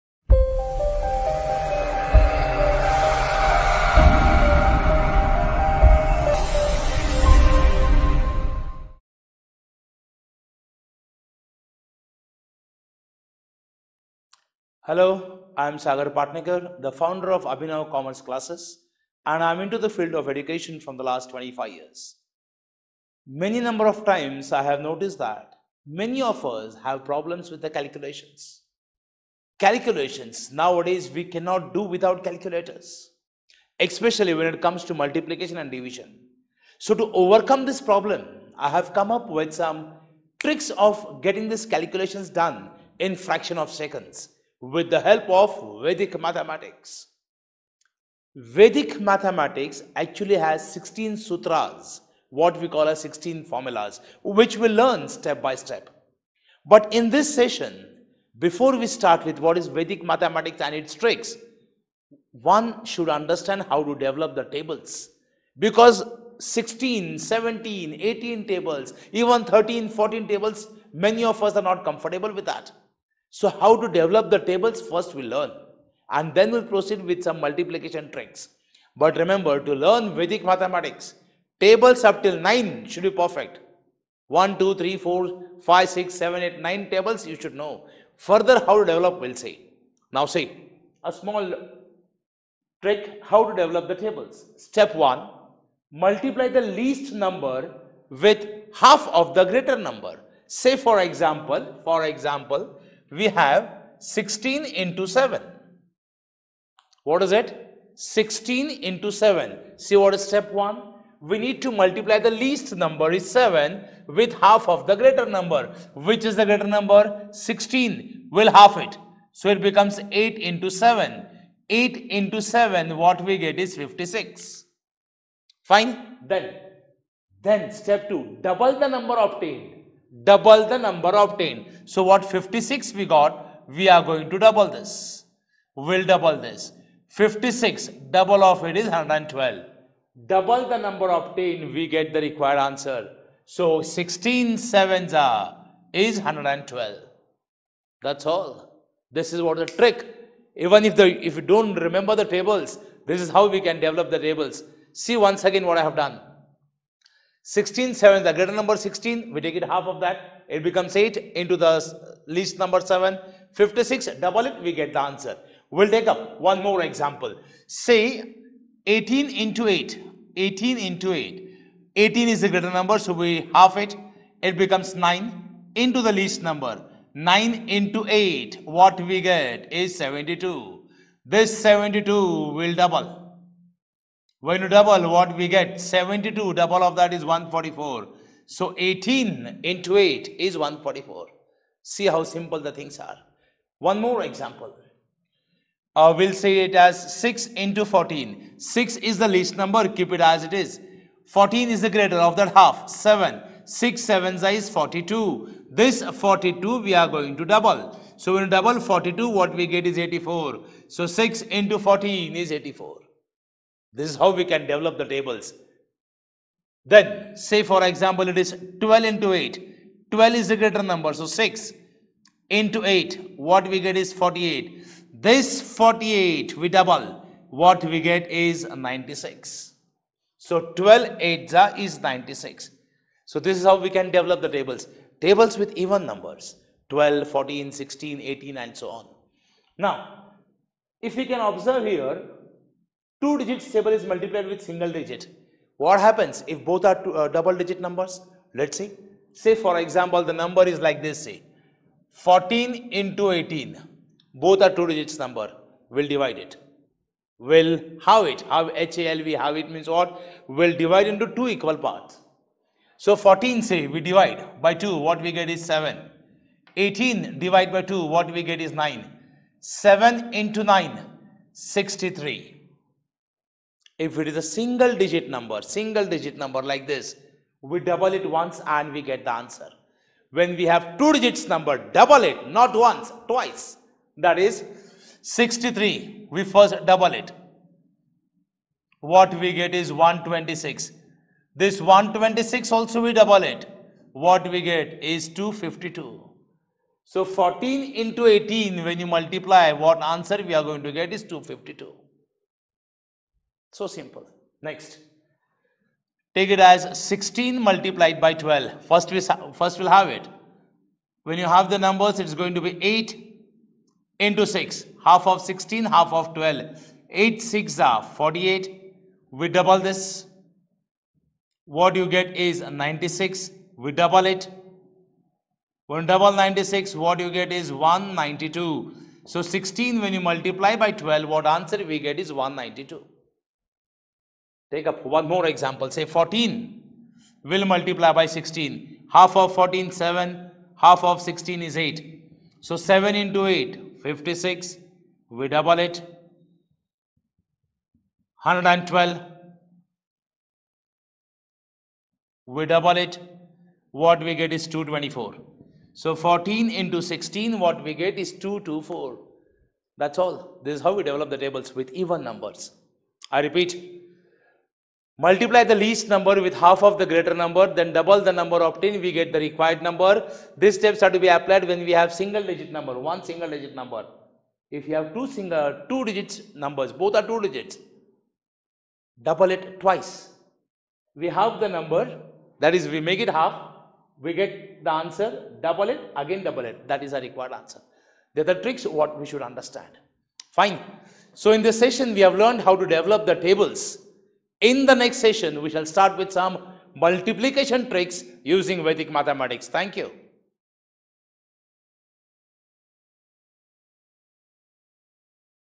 Developing Tables
Tables are essential for any calculations. In this video we will learn how to develop the tables in a fraction of a second.